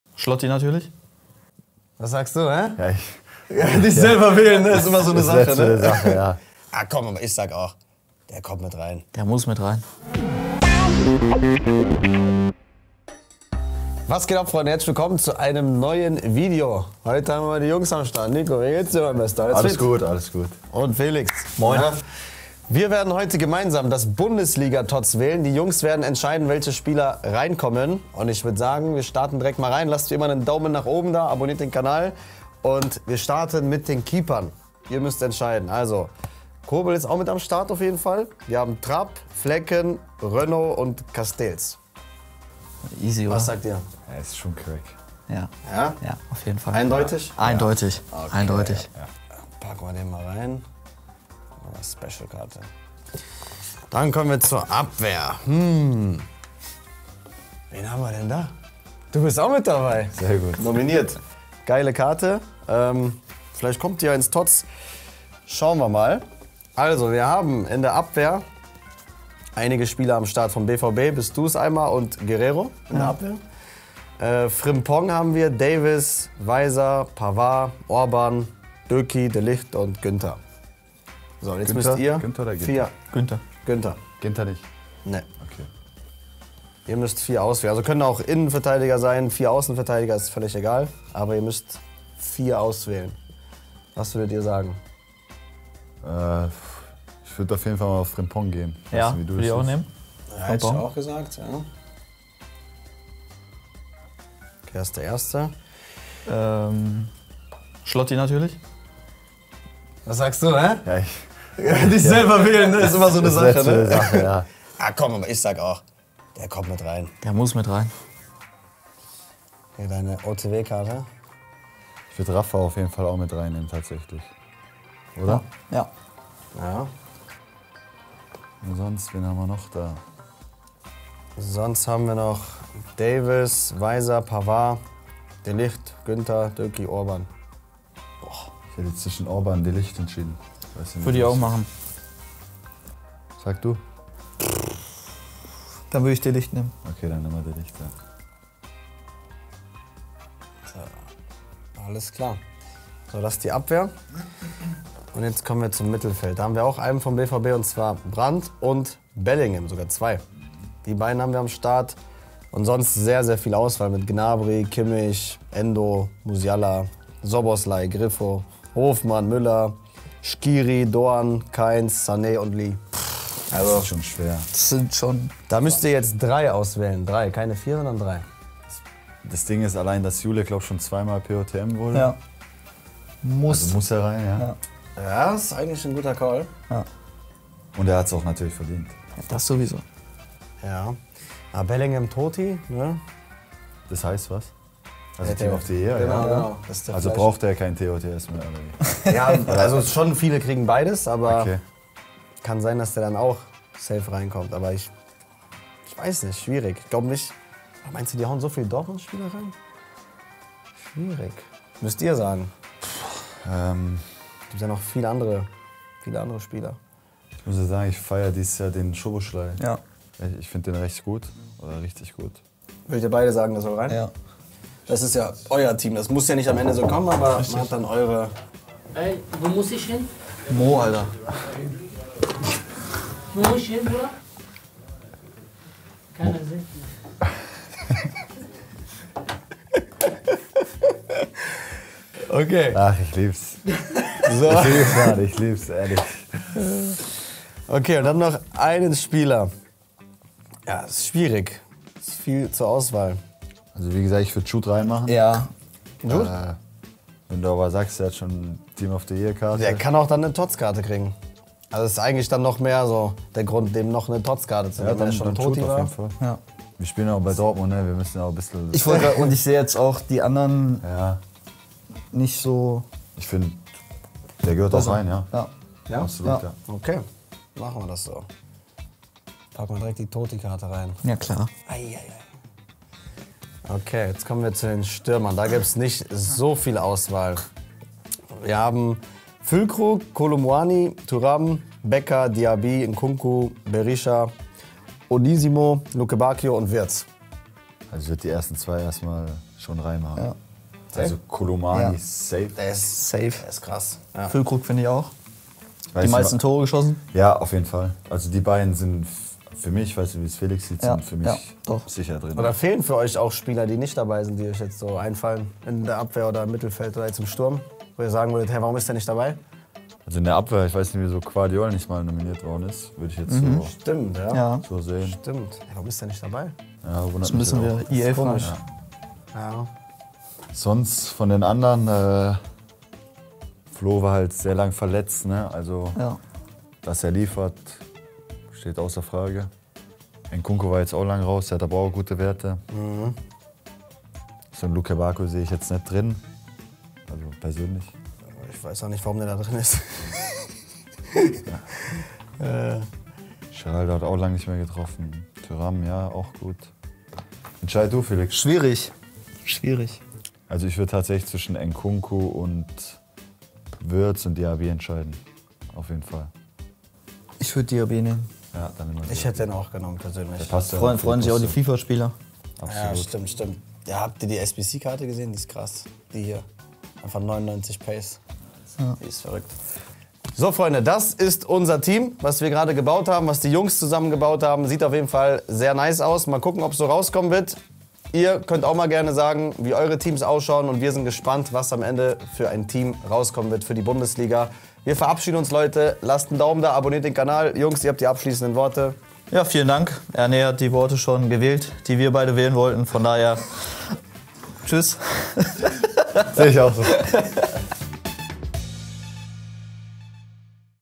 "He has to go in!" | Schlotterbeck & Passlack build their Bundesliga Team of the Season
Nico Schlotterbeck and Felix Passlack are building their Bundesliga Team of the Season with the help of Erné. Watch here, whether the nominated Schlotterbeck makes it into the selection, how many teammates are in total and who pays the two a small surprise visit at the shoot.